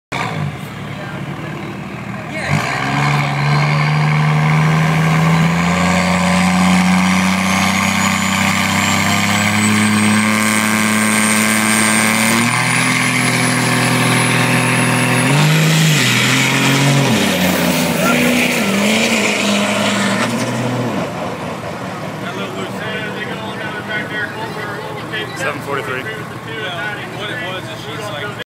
SDX '11
Endeavor blowing the tires off and getting out of it.